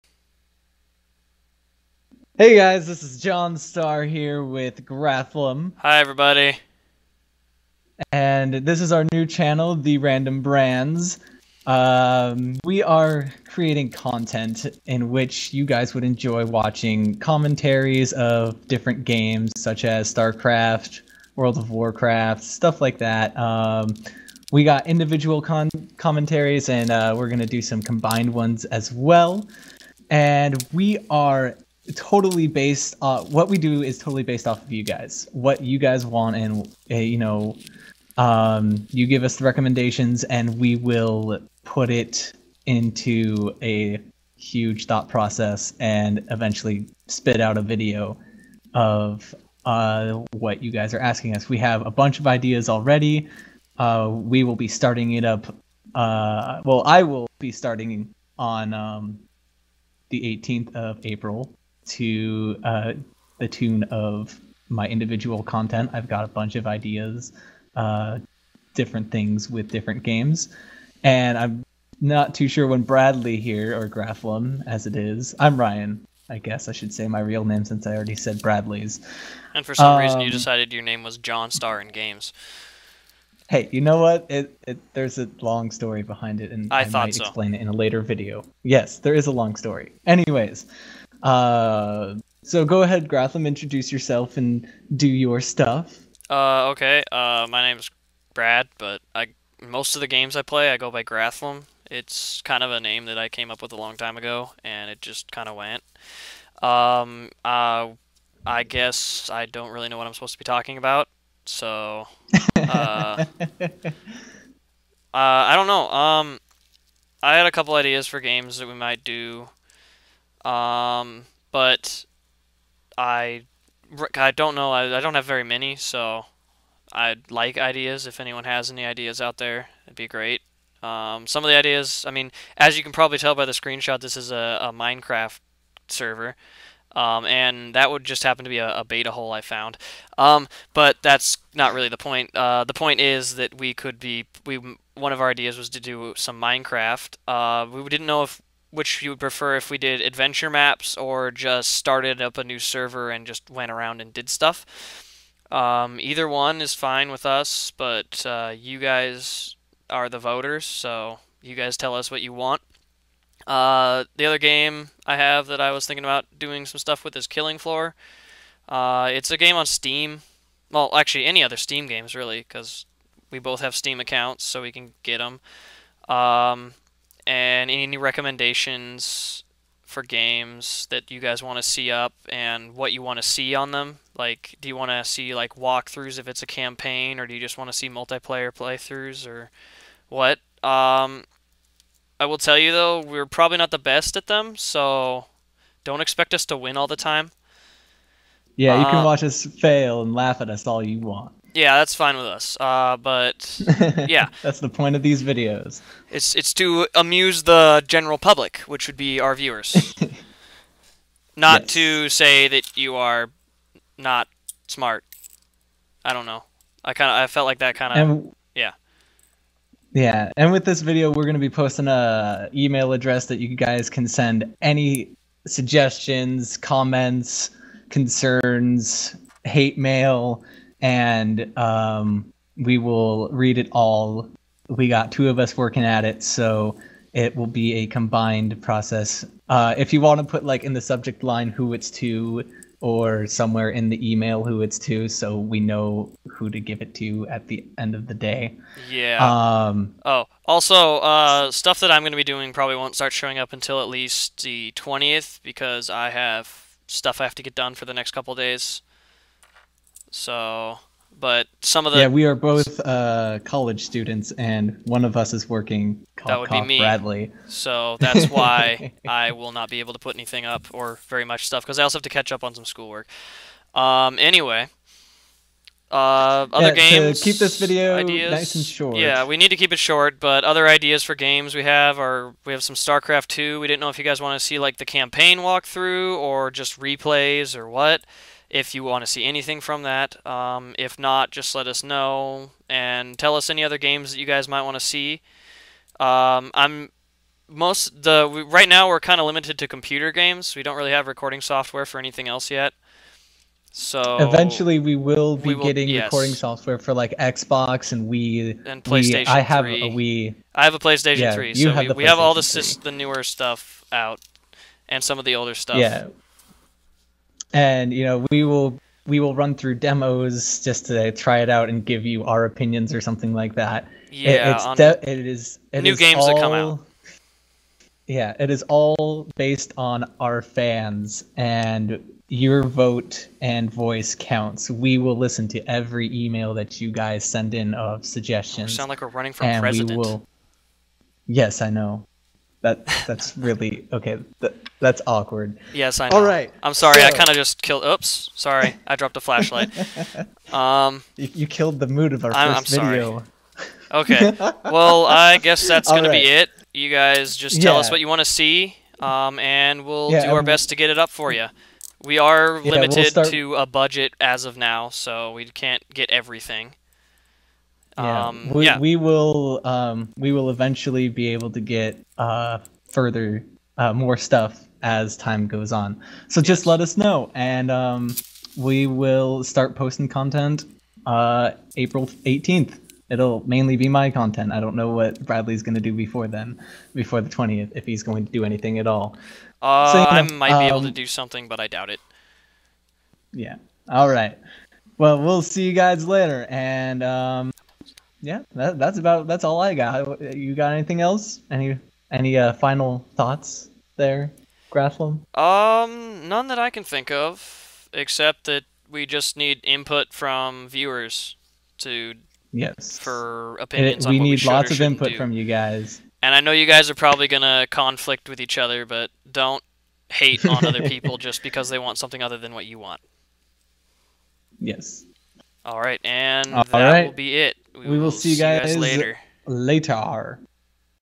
Introduction- about us and what our channel is about
This video is to introduce us to you and to tell you what this youtube channel is about.